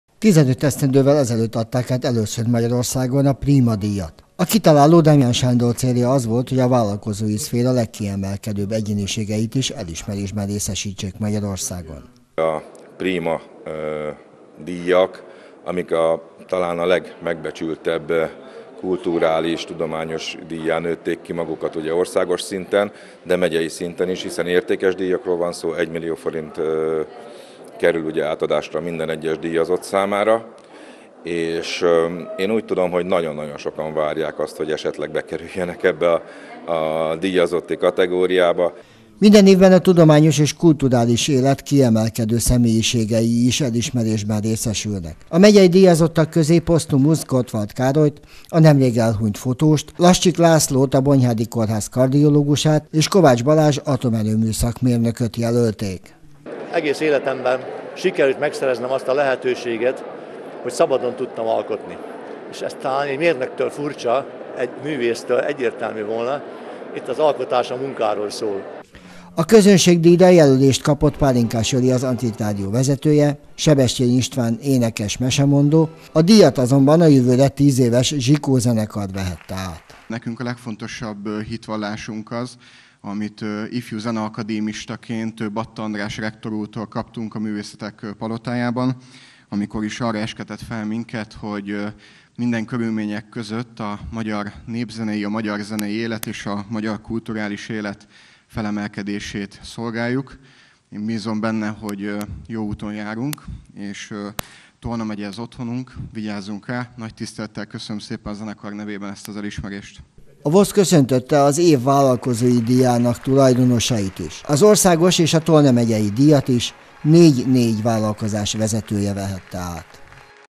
Tolna megyei Prima díj Kováts Balázs . Zsikó Zoltán 20191111
Idén is hárman vehették át a Tolna megyei Prima díjat. A Vállalkozók és Munkáltatók Országos Szövetsége a kiemelkedő teljesítményeket jutalmazza minden esztendőben. Kovács Pál, a Paksi Atomerőmű kapacitásának fenntartásáért felelős államtitkár Süli János miniszter üdvözletét tolmácsolva azt mondta: Tolna az egyik legkisebb megye, mégis nagyon sok tehetséget adott az országnak.